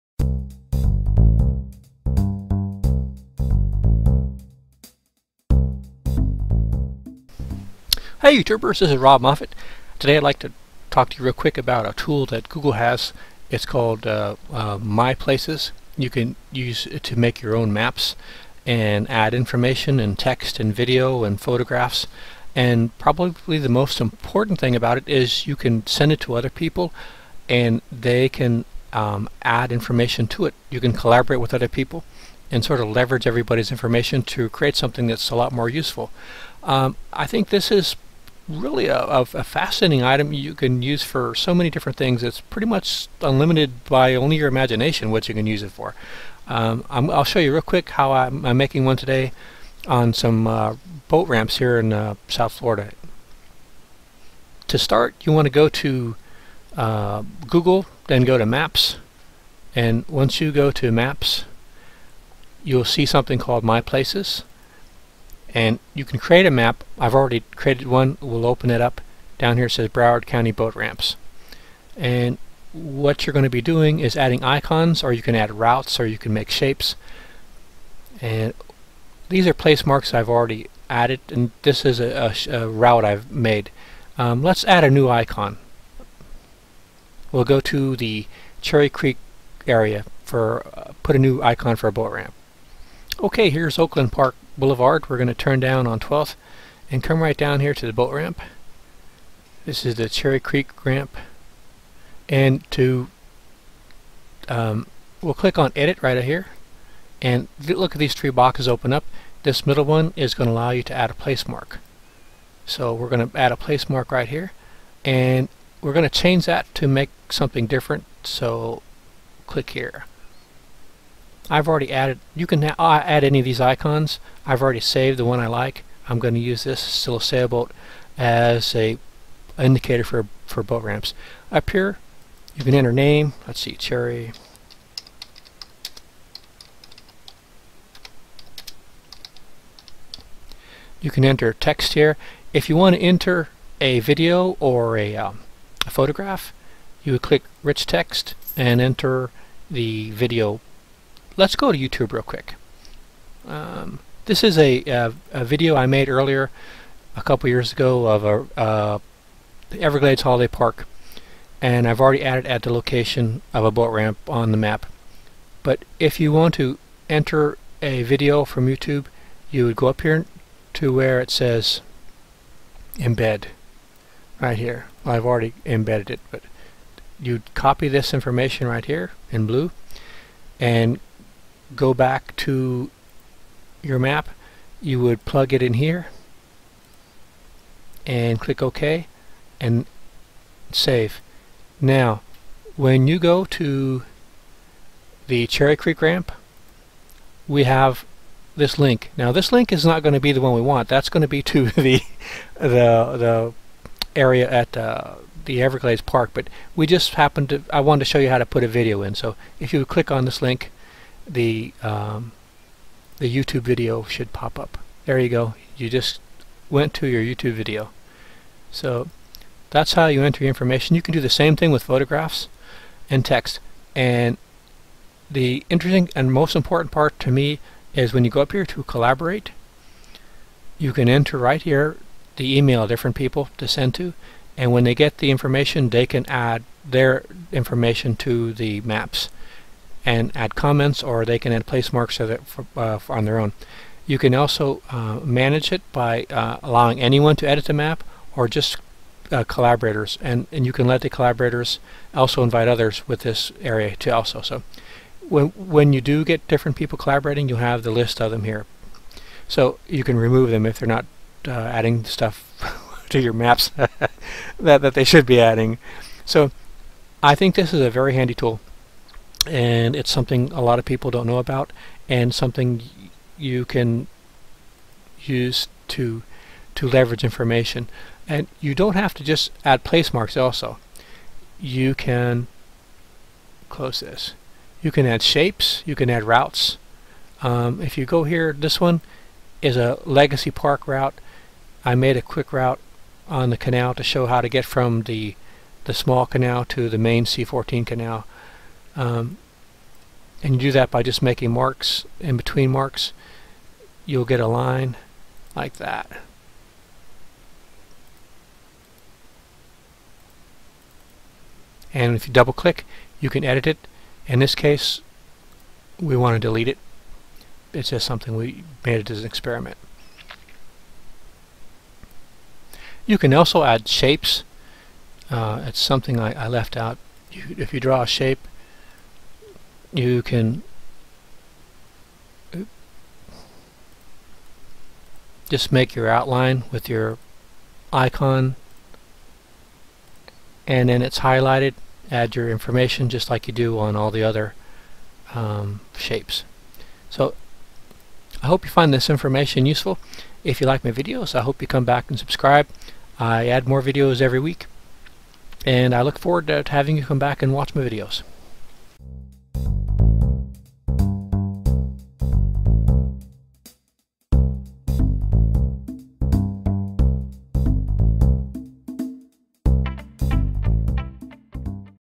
How to make your own My Maps with Google maps
How to create personal custom maps using Google Maps and the My Places tool..

The Google My Places is a very useful tool. My Places tool lets you not only make maps and add text and photos and video and routes, but if you click on the link button in the edit mode you will get a  url  that will let you  embed the map you made in a website. You can also email your map to collaborators who can then add their own information to make the map even more useful.

 I would like to see a local map that detailed the location of low cost health services, community gardens, community workshops,places to buy low cost fruit,meats and vegetables,historical maps made by long time local residents,fishing maps,job training maps, nature trails,and a lot more. Now that I have a tool like My Places, I can actually make those maps and share with others, ...and so can you.

If you make DIY or outdoor or low cost living or prepper videos, I would be happy to check them out.